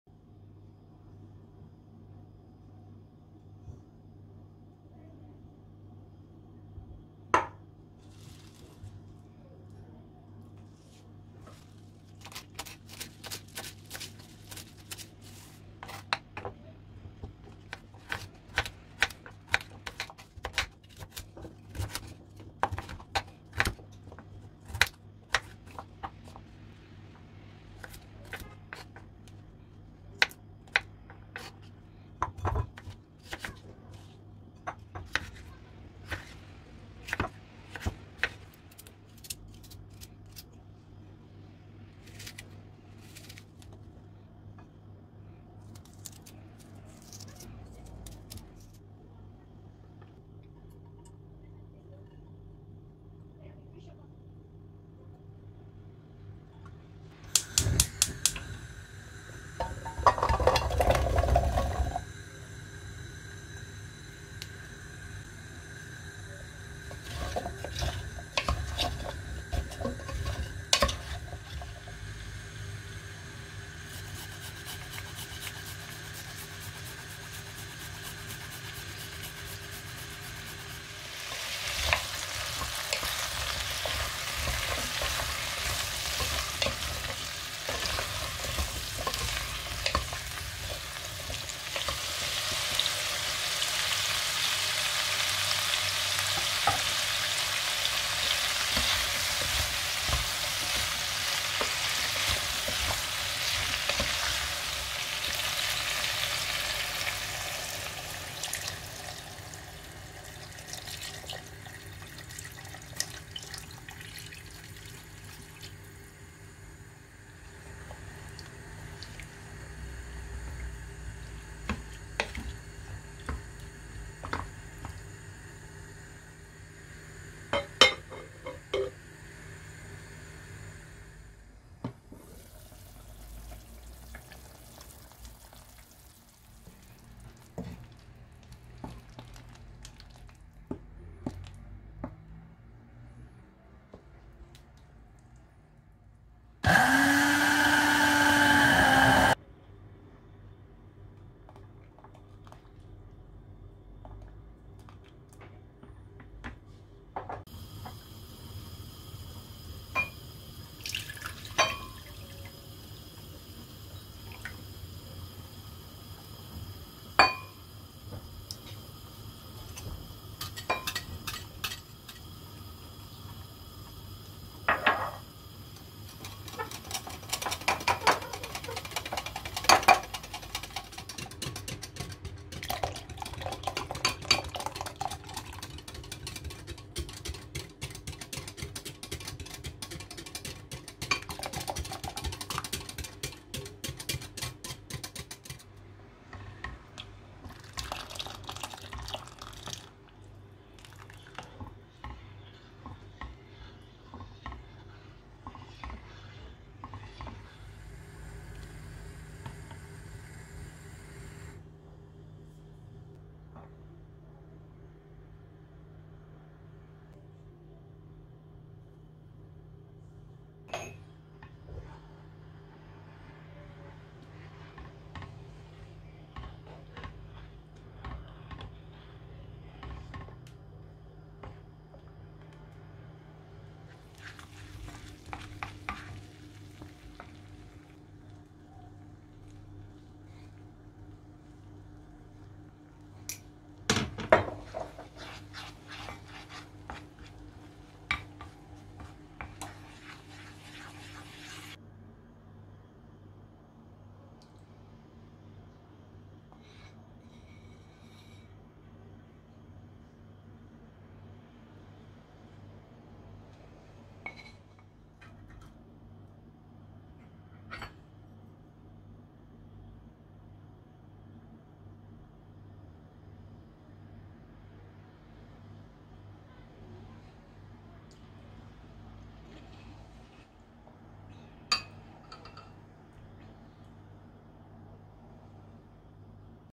Mis Gibi ve Çok Kolay! 😍 ŞİFA Deposu Taze Zencefilli Balkabağı Çorbası | EFSANE Kış Çorbası 🔥
Mis Gibi ve Çok Kolay! 😍 Şifa Deposu Taze Zencefilli Balkabağı Çorbası | EFSANE Kış Çorbası 🔥

♥  Altyazılar mevcuttur:(cc) , videoda etkinleştirebilirsiniz.

(♥ Subtitles are available:(cc) , you can enable it in the video.

MALZEMELER:
450 gr balkabağı
100 gr kereviz
1 orta boy havuç
1 orta boy patates
1 orta boy kuru soğan
3 diş sarımsak
1 Su bardağı süt
1 yumurta sarısı
2 yemek kaşığı un
Taze zencefil
Karabiber
Tuz
Su

Afiyet olsun 🤗❤